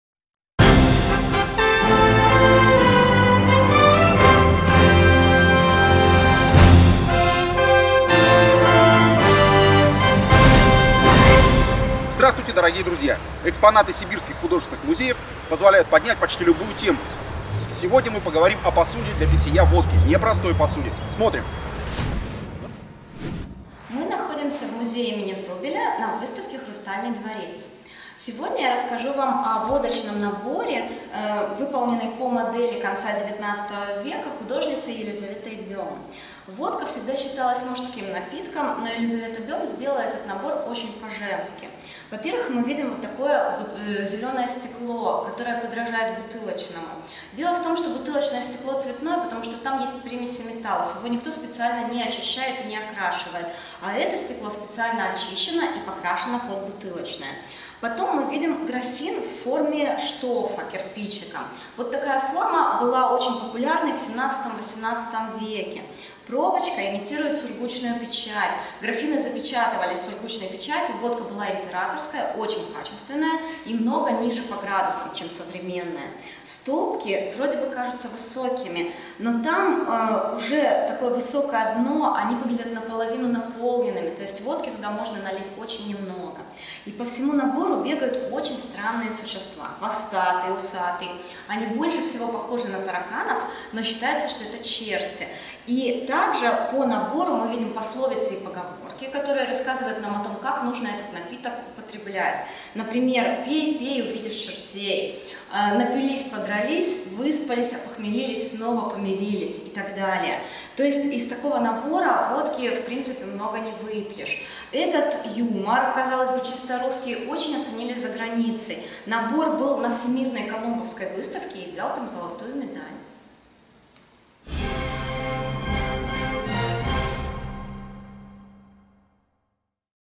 Набор для питья Водки в Генерал Губернаторском дворце. Омск в проекте Музеи Сибири.168
Набор для питья Водки в Генерал Губернаторском дворце. Омск в проекте Музеи Сибири.
Экспонаты сибирских художественных музеев позволяют поднять почти любую тему. Сегодня мы поговорим о посуде для пития водки. Не простой посуде.

В наших проектах "Музеи Сибири", "Музыка в музее", "Поэзия в музее" вы сможете увидеть много нового и интересного. Уникальные экспонаты. Талантливые музыканты, композиторы, поэты.